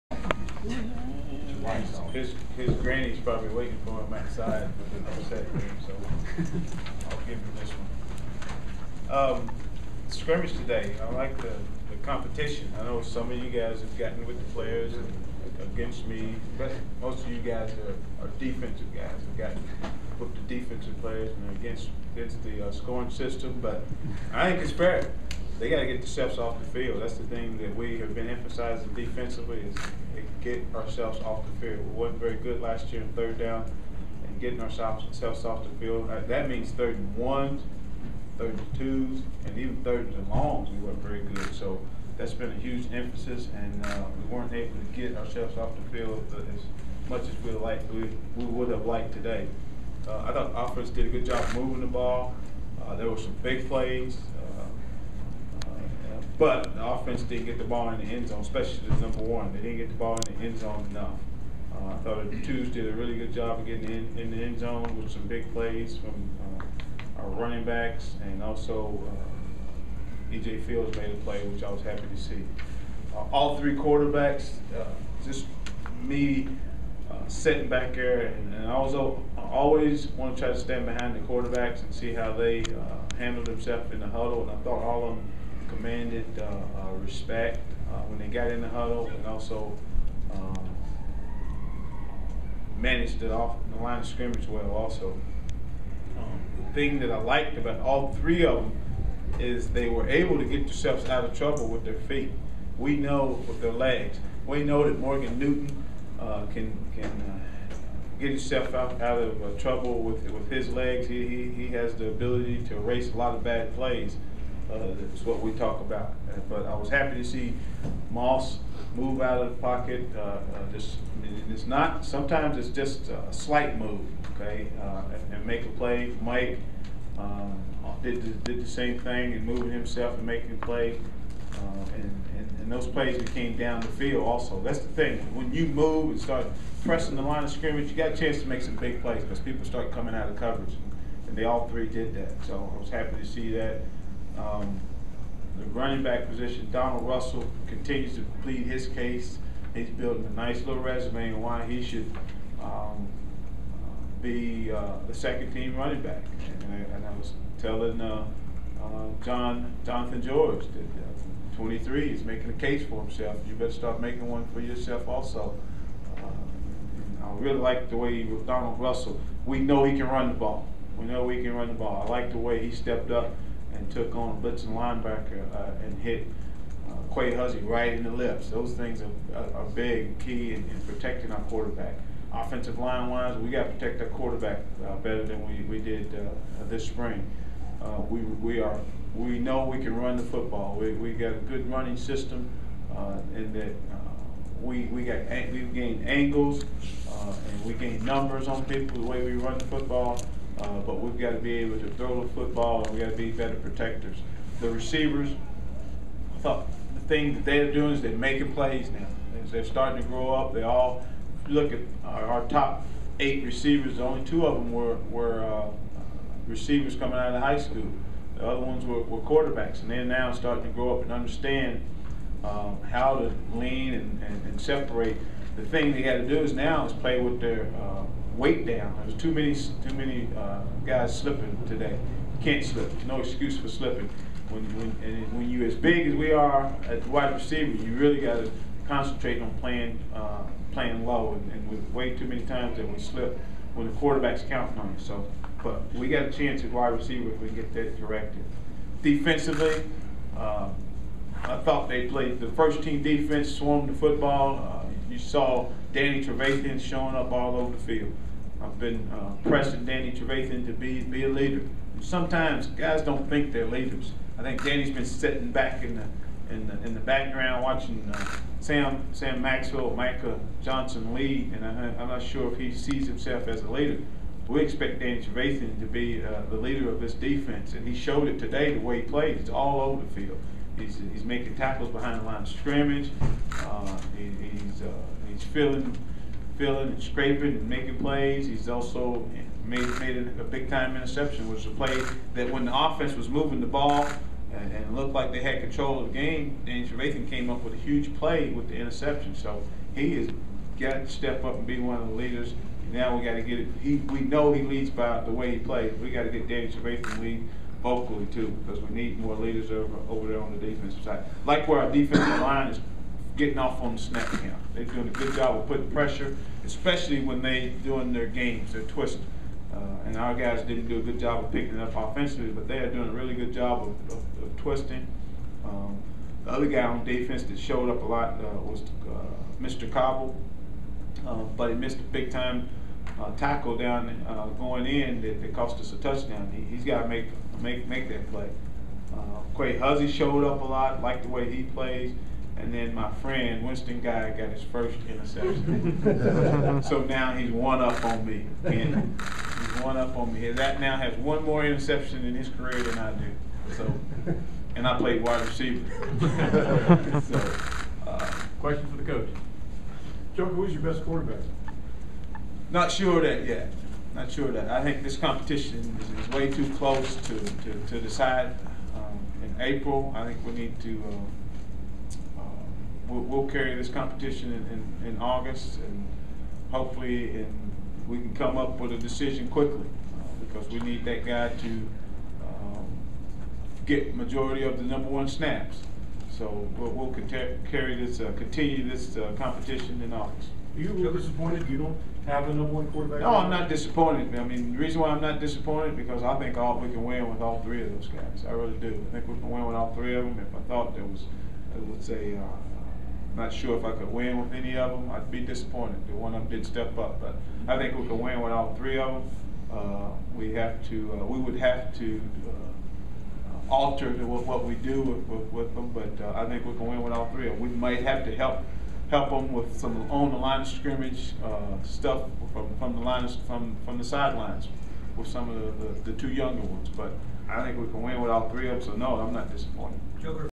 Joker Phillips shares his thoughts on the Blue/White game
UK head coach Joker Phillips discusses his team's performance in the Blue/White scrimmage.